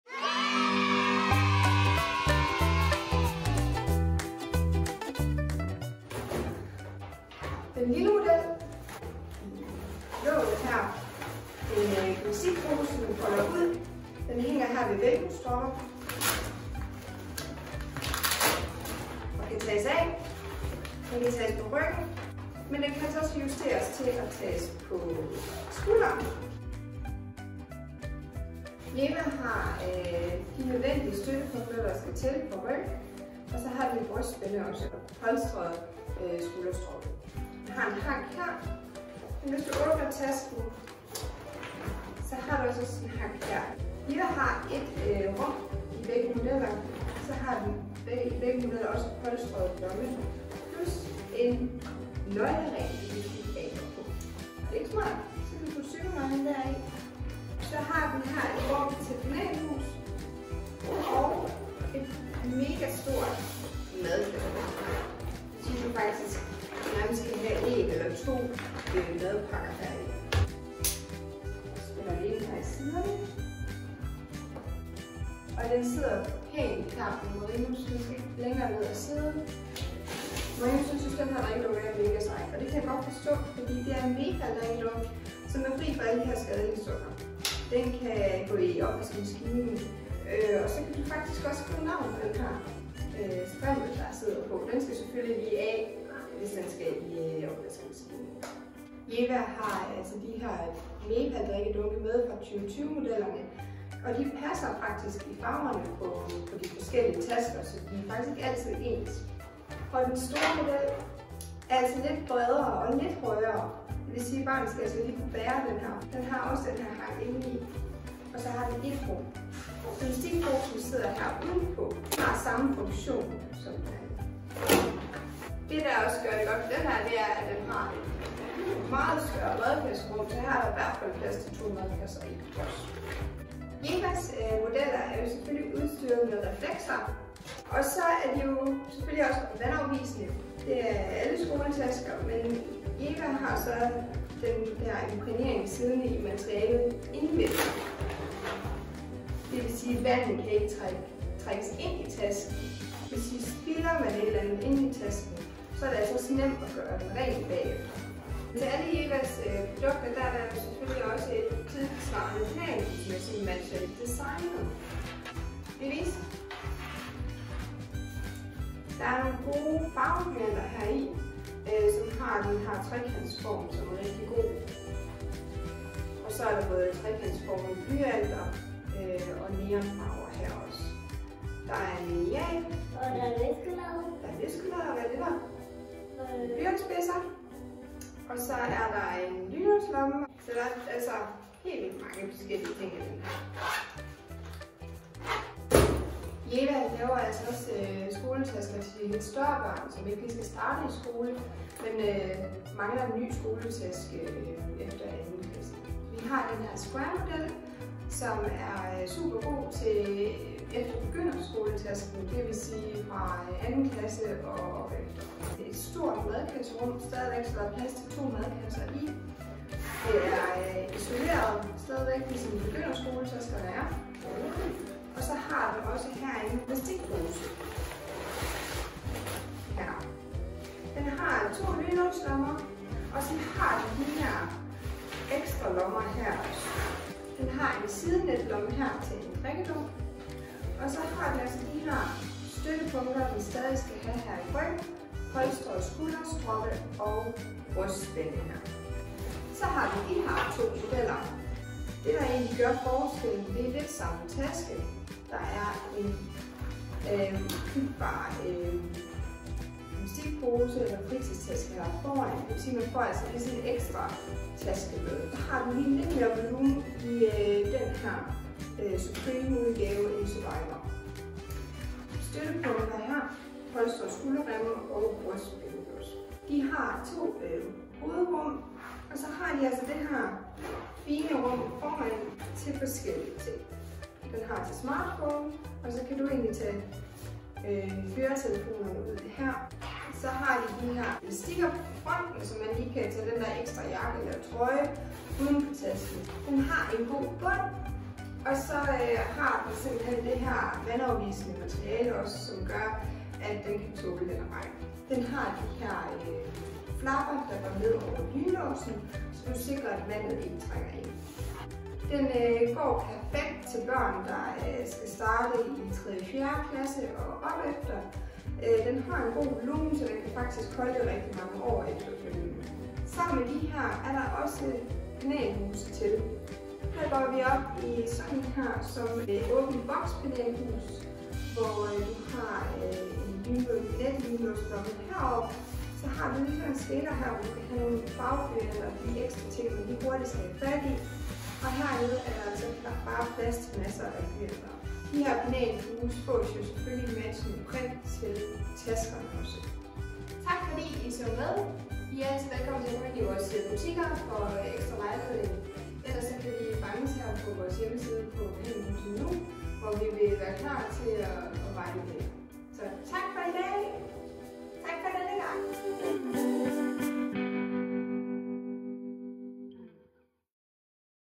Guide til Jeva skoletasker // Helm.nu
Jeva er et dansk mærke som vægter ergonomi meget højt. Her gennemgår vi Jeva's forskellige modeller helt fra 0. klasse og opefter.